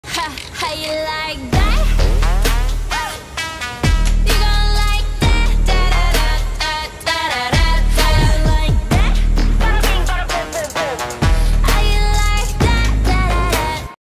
traveling with gb120 mobility scooter
Exiting the plane and getting the scooter ready to ride to the next plane